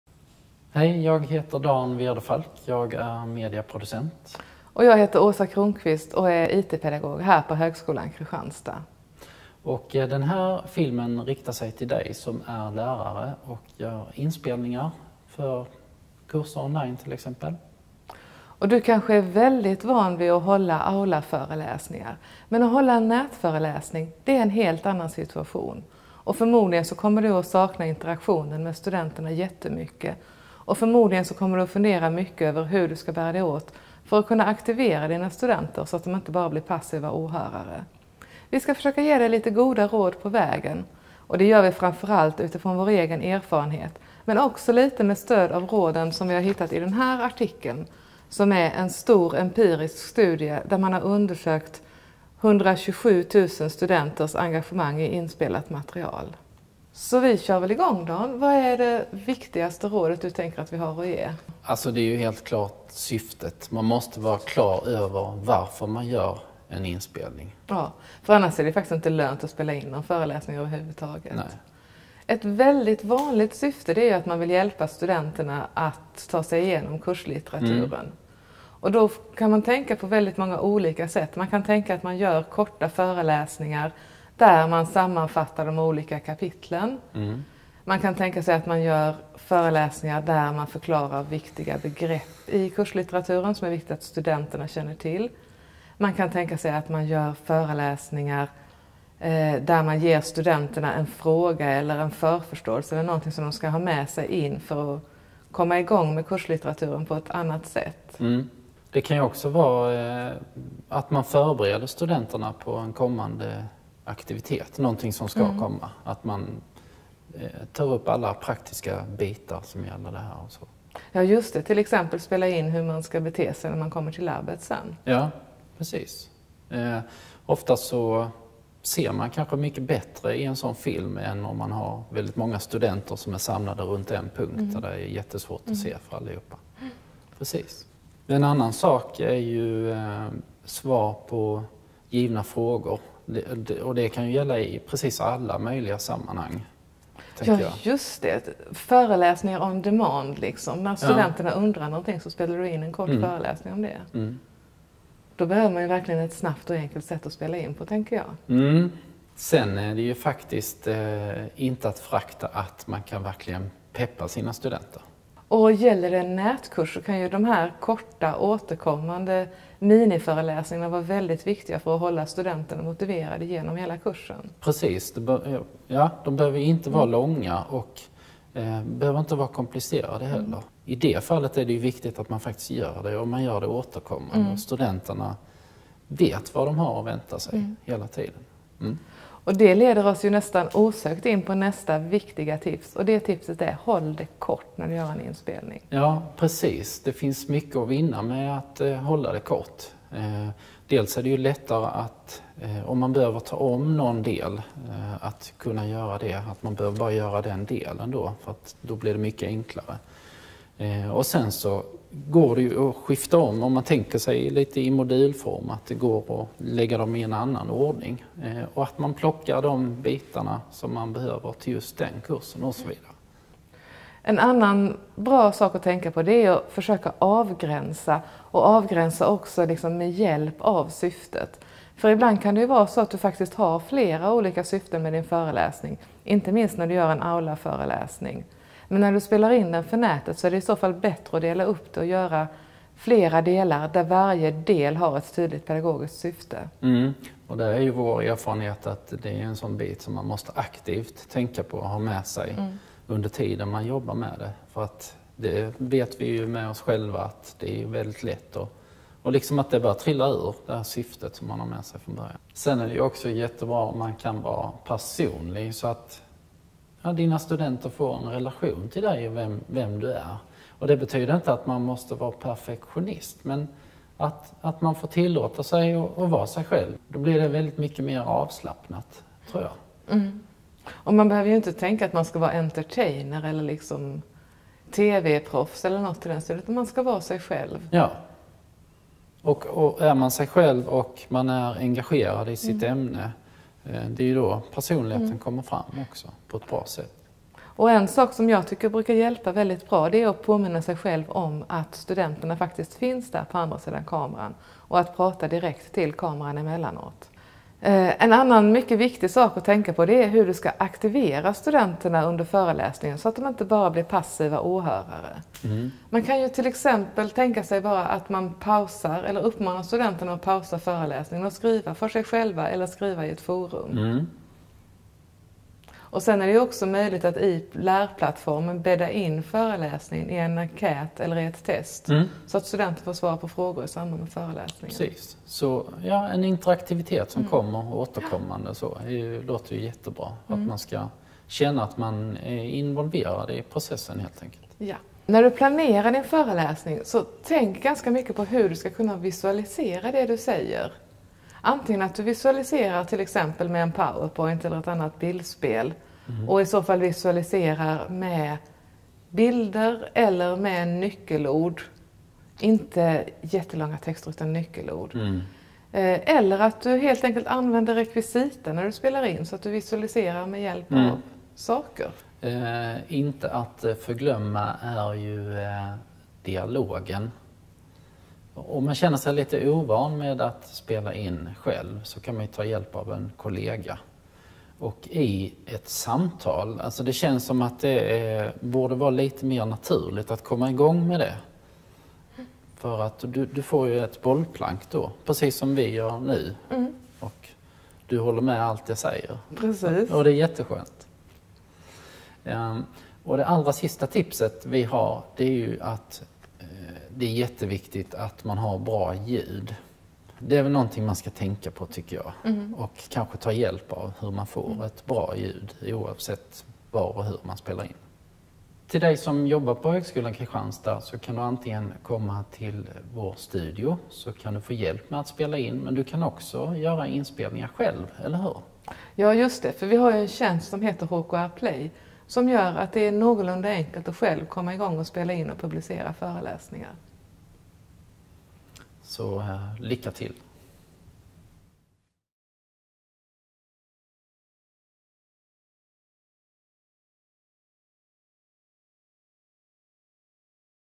Mot bättre nätföreläsningar
Ett samtal om vad du kan tänka på när du planerar en nätföreläsning. Den artikel som nämns i filmen hittar du på

Producerad av Åsa Kronkvist och Dan Wirdefalk, LärandeResursCentrum, Högskolan Kristianstad